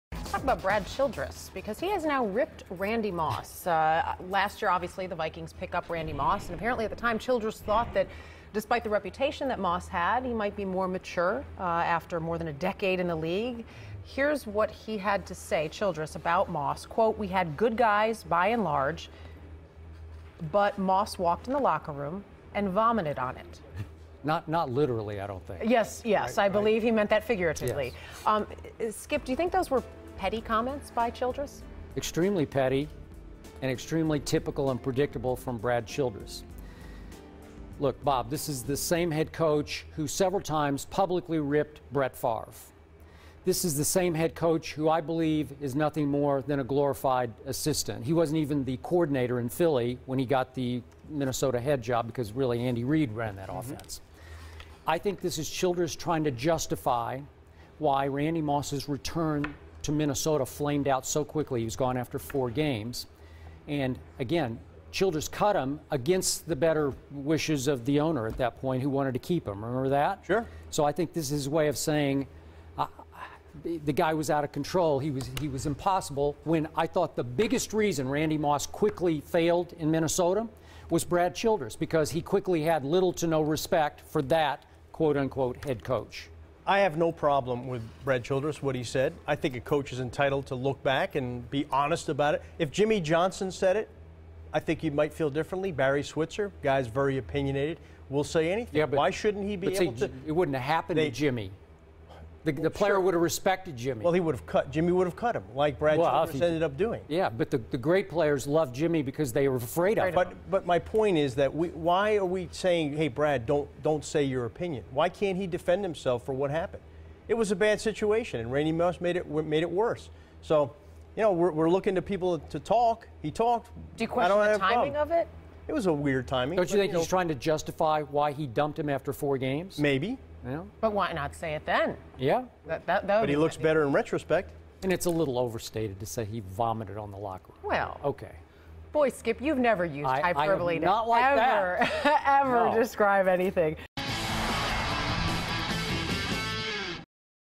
Brad Childress Rips Randy Moss
Bob Glauber and Skip Bayless discuss Brad Childress' comments about Randy Moss. Copyright ESPN